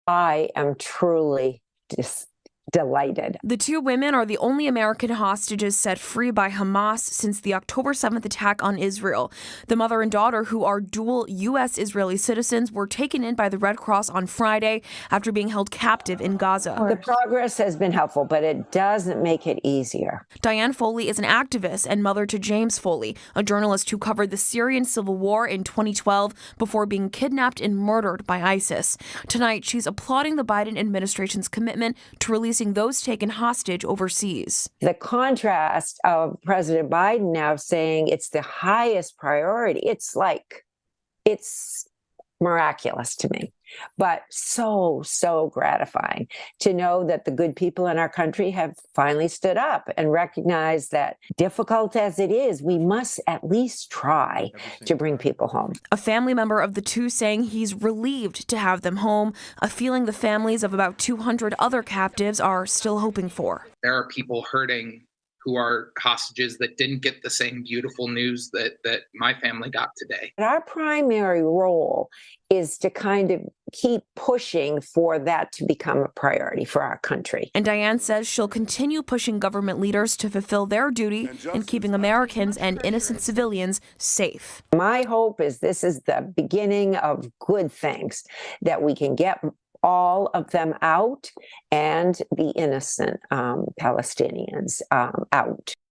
Local advocate hopes for more negotiations to release American hostages held by Hamas
After hearing the news that Natalie and Judith Raanan were released, founder of the James W. Foley Legacy said she applauds the Biden Administration's commitment to releasing those taken overseas.